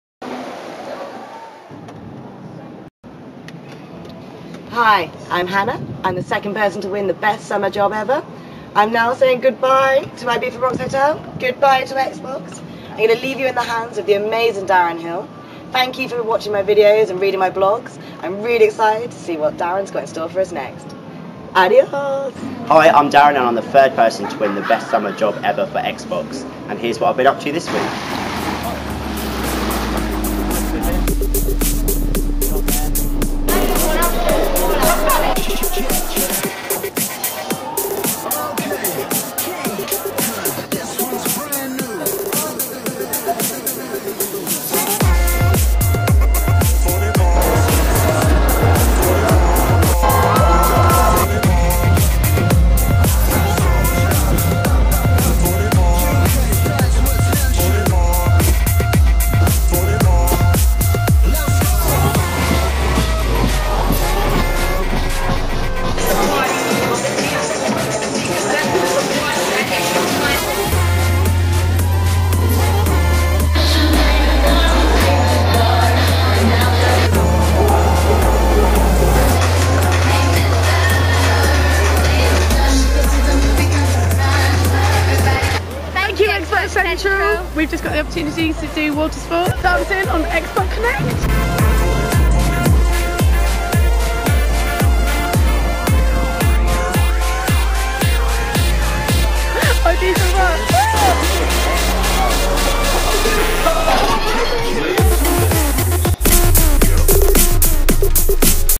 Darren Hill's First Video Update from Ibiza Rocks
Meet Darren Hill, the third and final winner of our 'Best Summer Job Ever' campaign, he'll be spending six weeks in Ibiza being our Xbox correspondant at the legendary Ibiza Rocks Hotel...